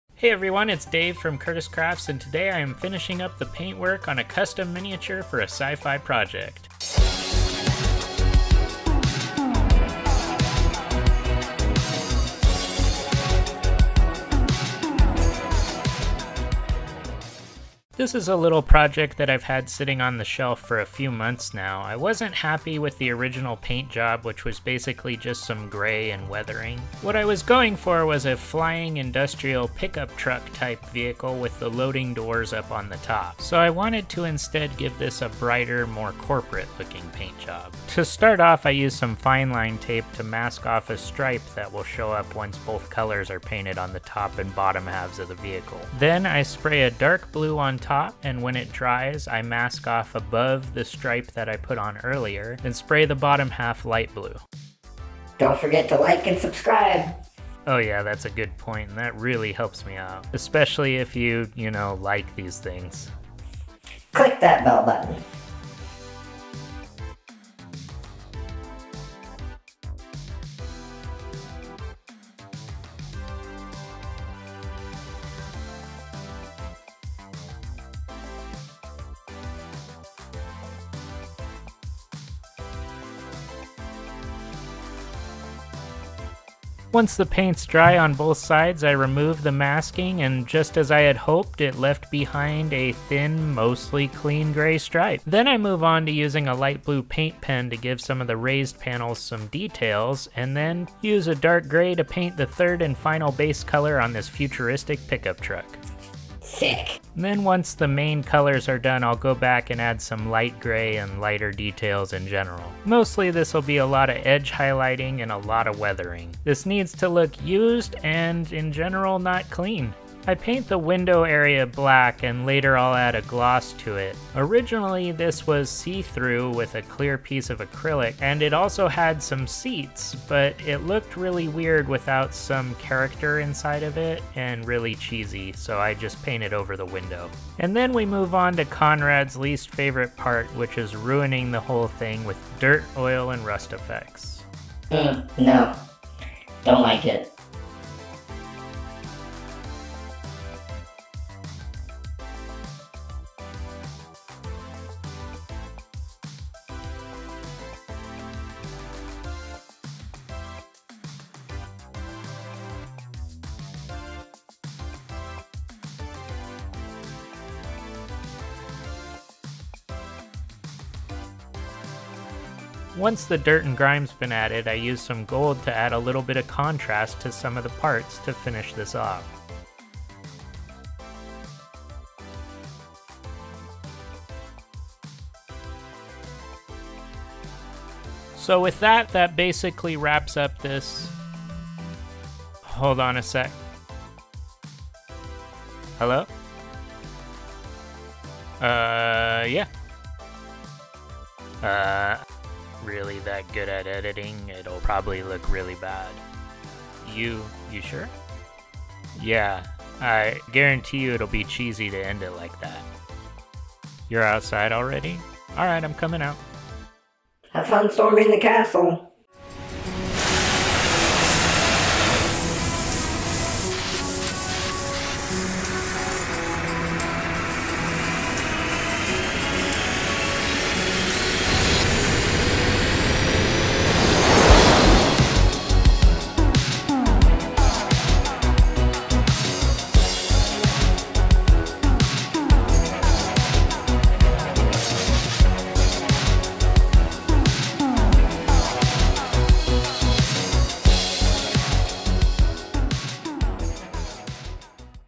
Painting a Scratch-Built Sci-Fi Ship for Green Screen
In this video I dust off an old project: a sci-fi miniature scratch build of a futuristic pickup truck. It was originally just gray and rusty and weathered and there just wasn't any character to it. Now it's much better, and will be cool if I ever decide to make a cheesy sci-fi movie.

This is mostly focused on the painting because I made this project before I ever started filming my work. But it's a good re-visit.